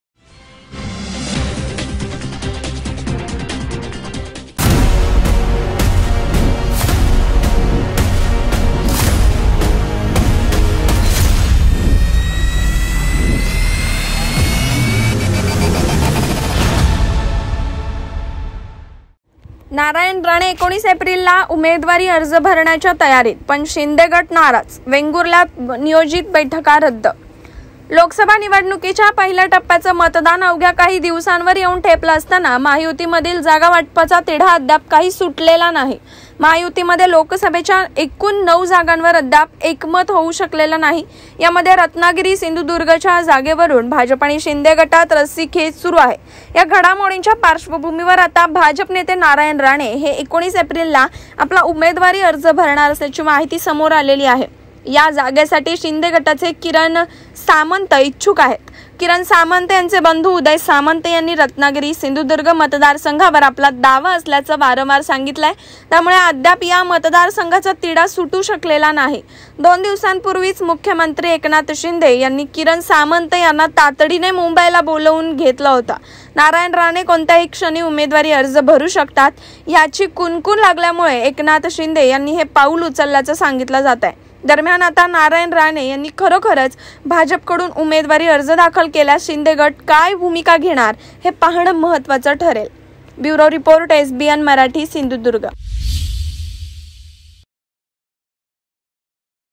SINDHUDURG |नारायण राणे 19 एप्रिलला उमेदवारी अर्ज भरण्याच्या तयारीत
नारायण राणे 19 एप्रिलला उमेदवारी अर्ज भरण्याच्या तयारीत, पण शिंदे गट नाराज; वेंगुर्ल्यात नियोजित बैठका रद्द.